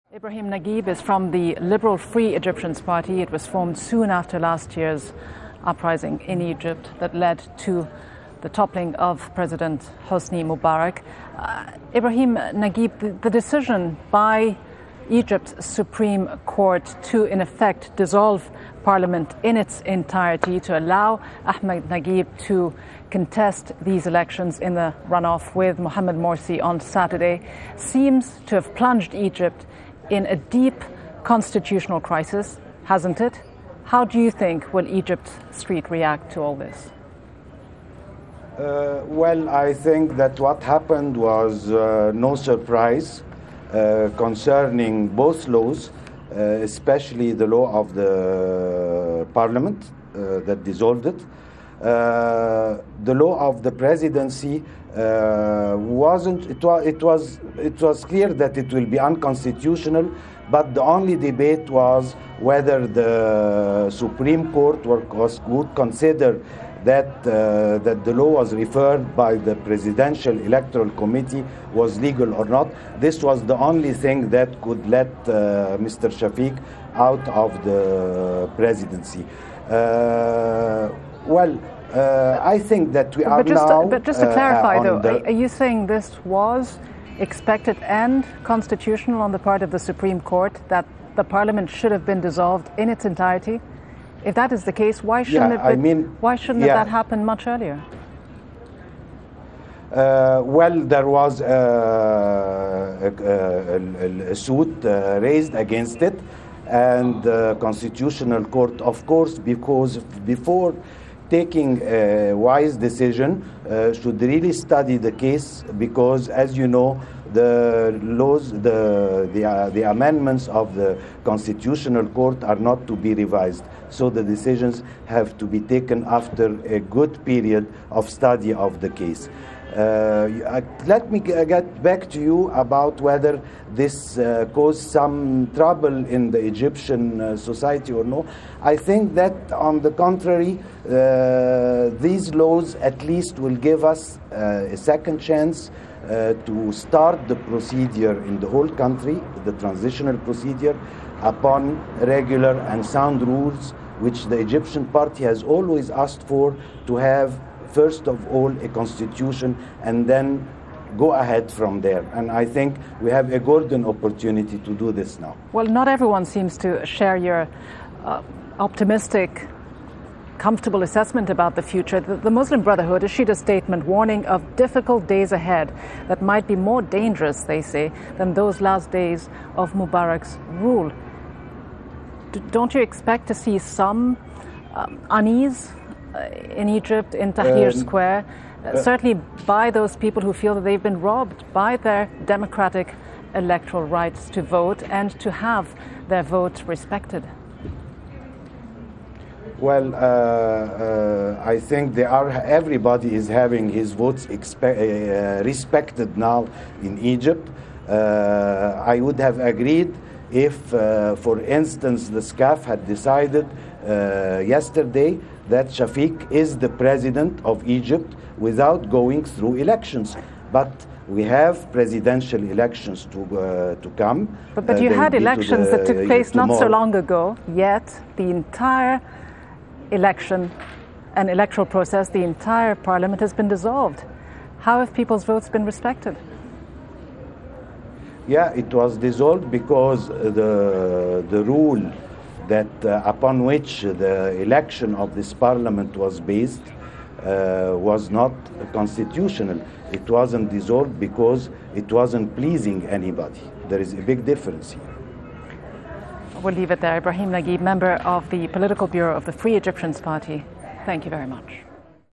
Ibrahim Naguib
Ibrahim Naguib, from the liberal Free Egyptians party, talks about recent political developments.